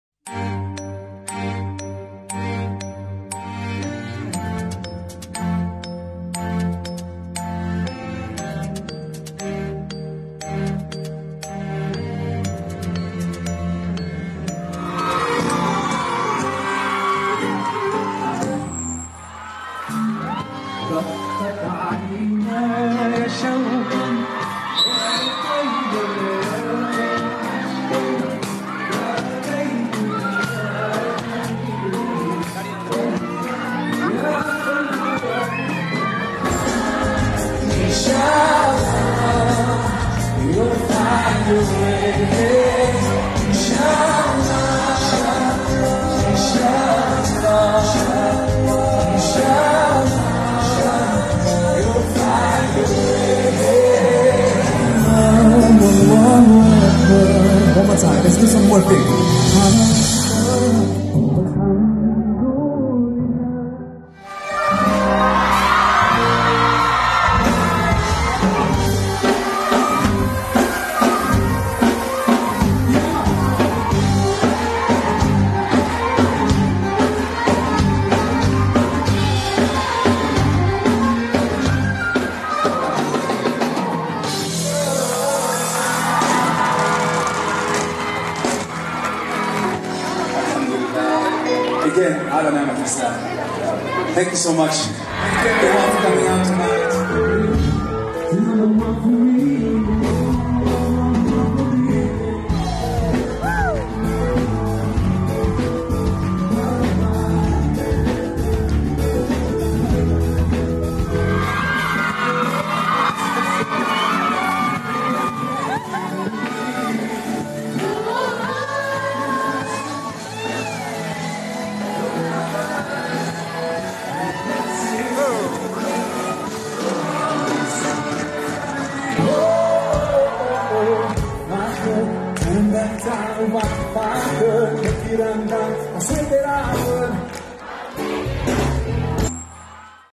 Maher Zain concert at Tanzania
Maher Zain Concert in Tanzania some time ago 😍💚